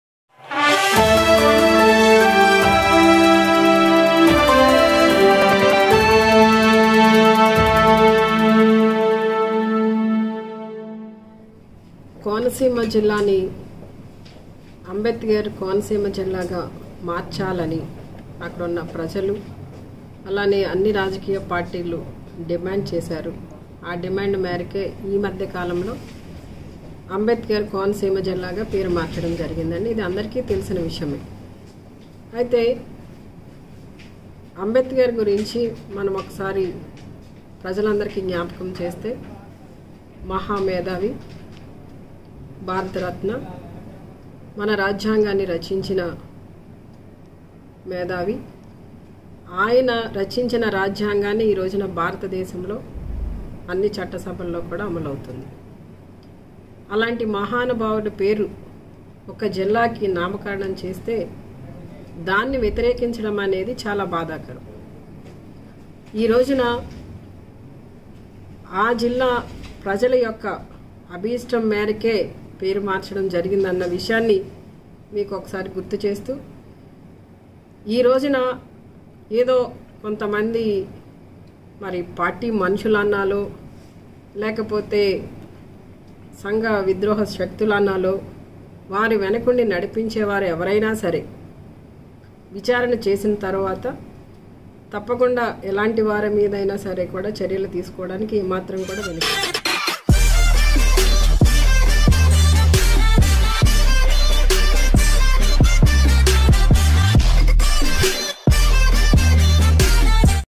ఆందోళనకారులు, వారి వెనుక ఉండి నడిపించే వారిపై చర్యలు...హోంమంత్రి
*బ్రేకింగ్ పాయింట్స్...*

*మీడియాతో మాట్లాడిన  శ్రీమతి తానేటి వనిత - హోం శాఖ మాత్యులు...*

కోనసీమ జిల్లాను అంబేద్కర్ కోనసీమ జిల్లా గా మార్చాలని స్థానిక ప్రజలు, అన్ని వర్గాలు, పార్టీలు డిమాండ్ చేసాయి.

ఈ నేపథ్యంలో అంబేద్కర్ కోనసీమ జిల్లాగా ఈ మధ్యనే పేరు మార్చడం జరిగింది.

డా.బీఆర్ అంబేద్కర్ మహా మేధావి, భారత రాజ్యాంగ నిర్మాత, భారతరత్న,, ఆయన ఎంతో మందికి స్ఫూర్తి దాయకం.

అలాంటి మహానుభావుని పేరును ఒక జిల్లాకు నామకరణం చేయడాన్ని వ్యతిరేకించడం బాధాకరం.

కోనసీమ జిల్లా ప్రజల అభీష్టం మేరకే అంబేద్కర్ కోనసీమ గా పేరు మార్చిన విషయాన్ని ప్రతి ఒక్కరూ గుర్తించాలి.

కొందరు ఉద్దేశపూర్వకంగా గొడవలు, అల్లర్లు సృష్టించాలని చూస్తున్నారు.

గొడవలు చేసే వారి వెనుక ఉండి నడిపించే వారిని గుర్తించి చర్యలు తీసుకోవడం జరుగుతుంది.

ఈ రోజు 20 మందికి పైగా పోలీసులపై రాళ్లు రువ్వి గాయపరిచారు. స్కూల్ బస్సు ల ను కూడా తగులబెట్టారు.

శాంతి భద్రతలకు విఘాతం కలిగించాలని కొన్ని శక్తులు కుట్ర పూరితంగా  ప్రయత్నిస్తున్నాయి.

అలాంటి వారిని గుర్తించి కఠిన చర్యలు తీసుకోవడం జరుగుతుంది.

పోలీసులపై జరిగిన దాడిని ఖండిస్తున్నాను.

ప్రజలను ఇబ్బందులకు గురిచేస్తున్న వారిపై తప్పక చర్యలు ఉంటాయి.

ఆందోళనకారులను, వారి వెనుక ఉండి నడిపించే వారిపై చట్ట ప్రకారం చర్యలు తీసుకోవాలని ఉన్నతాధికారులను అదేశిస్తున్నాను.